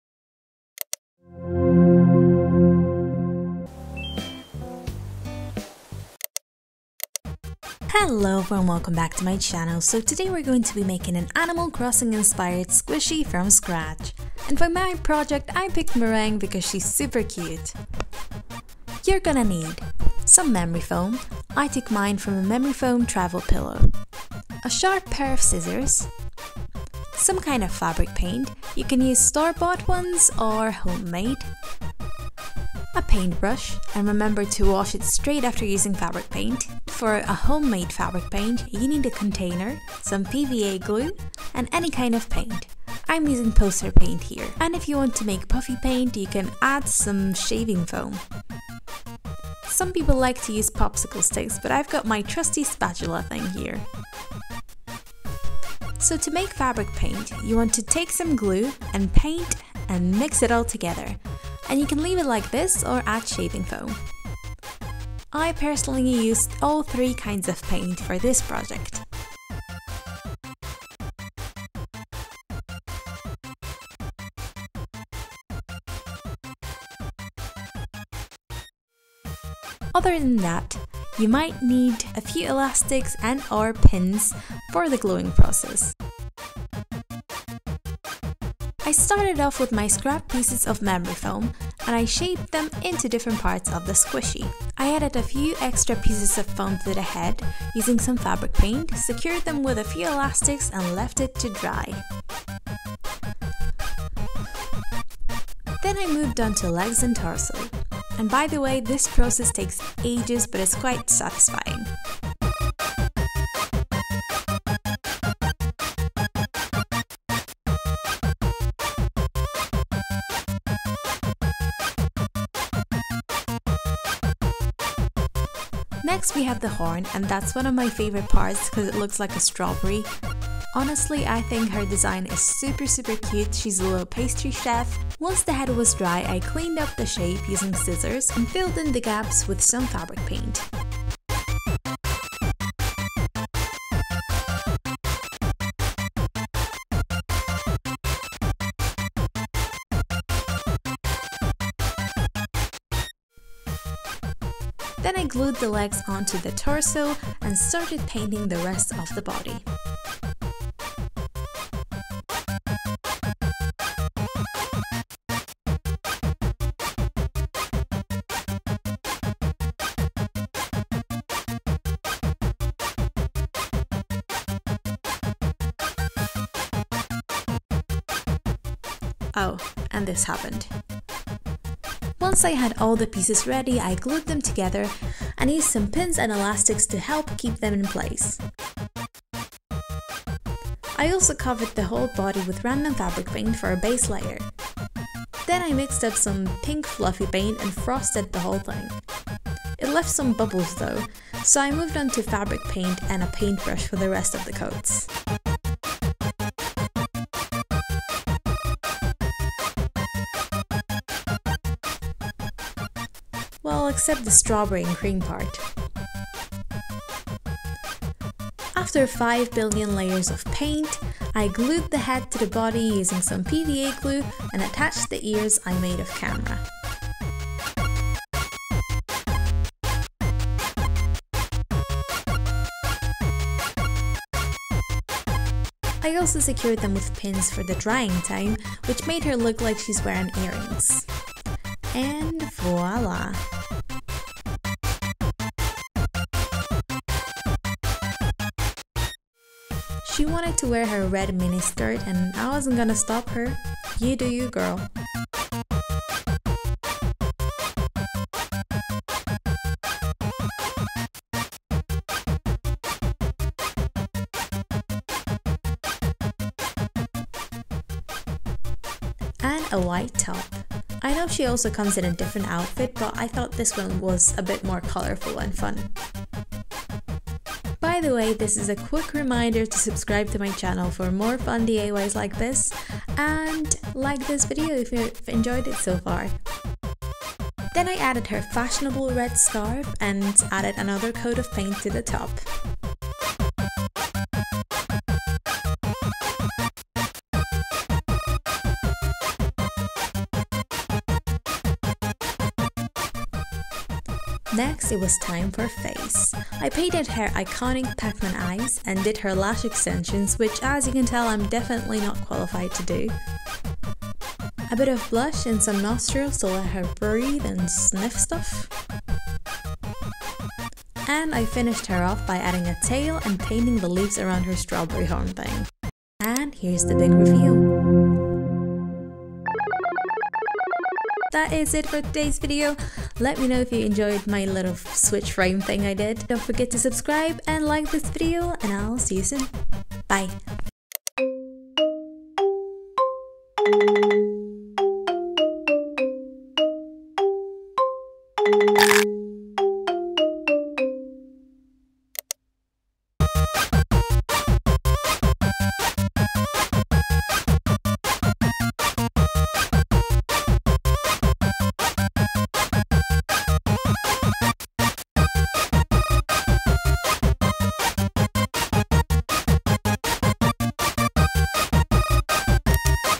Animal Crossing: Merengue Squishy DIY | How to Make a Squishy
Today I'm going to show you how to make your own adorable Merangue squishy. She's one of my favourite animal crossing villagers with her adorable design and the fact she's a pastry chef! (I love me some pastries)

Please let me know what you think about this weeks video and if you liked the little frame I added (took some time to draw hehe) or if it takes up too much space.

Who's your favourite villager?
Would you like to see more animal crossing squishies?

Materials:
-memory foam (from a travel pillow)
-scissors
-paintbrush
-Fabric paint (arteza and homemade)
-pva glue
-elastics and pins